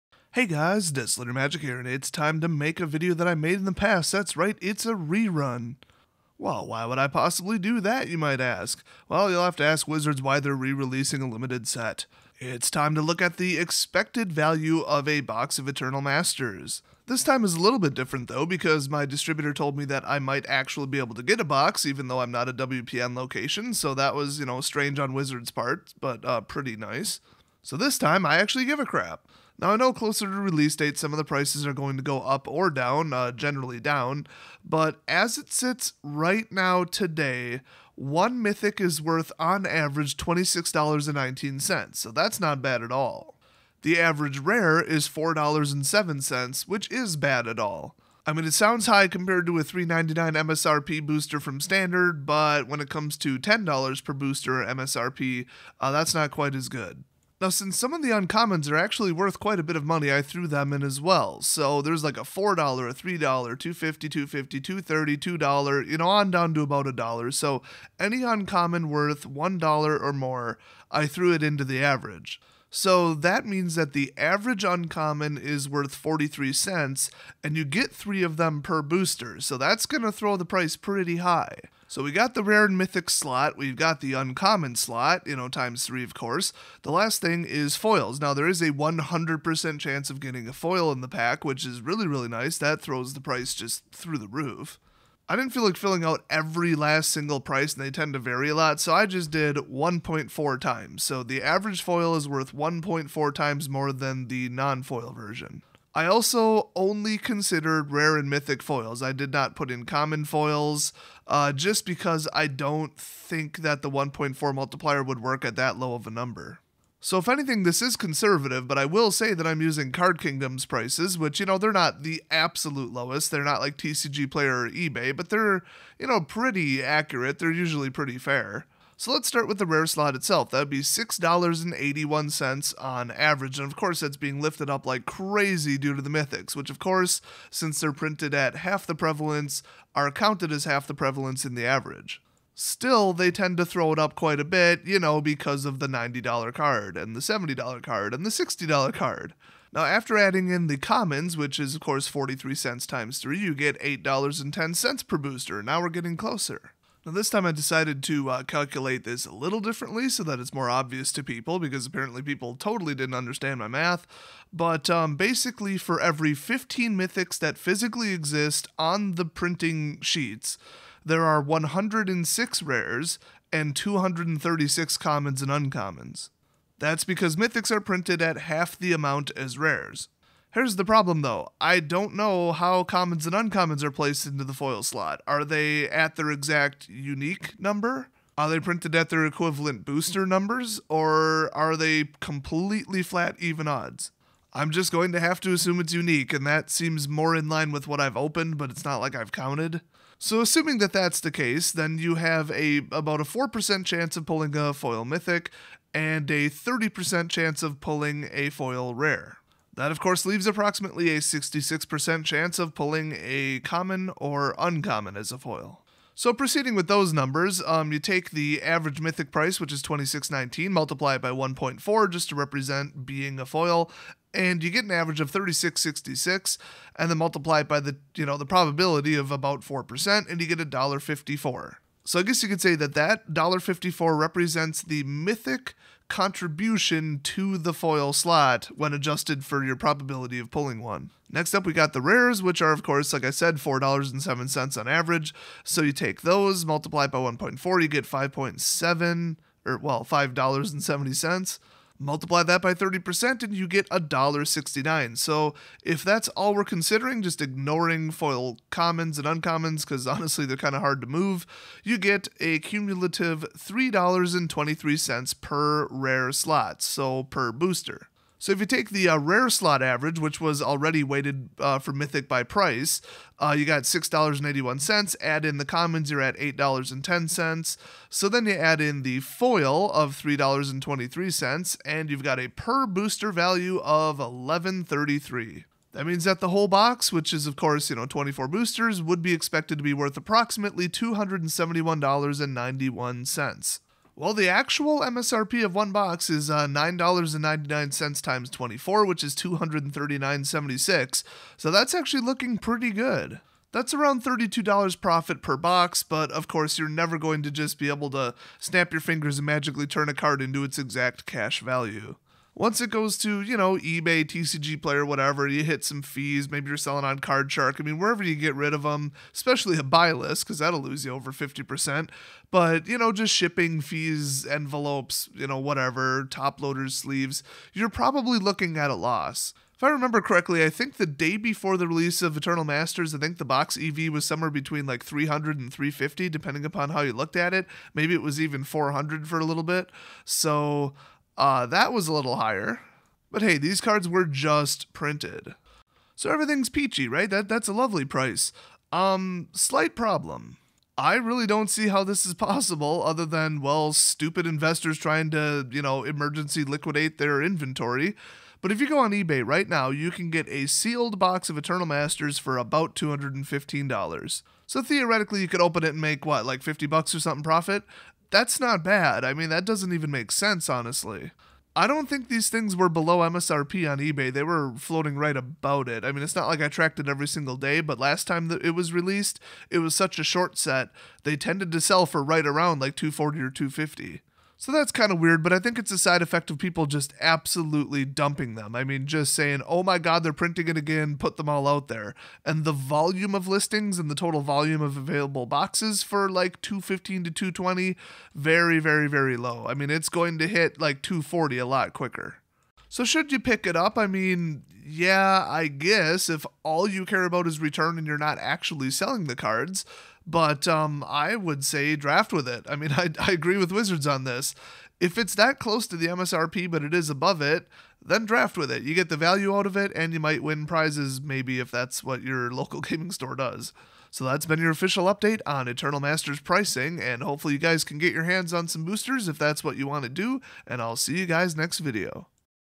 What's the Current EV of a Box of Eternal Masters?
The "reprint" of EMA will be releasing soon so the question on everyone's mind is what is the current EV of a box?  I did all the math and was surprised by the results.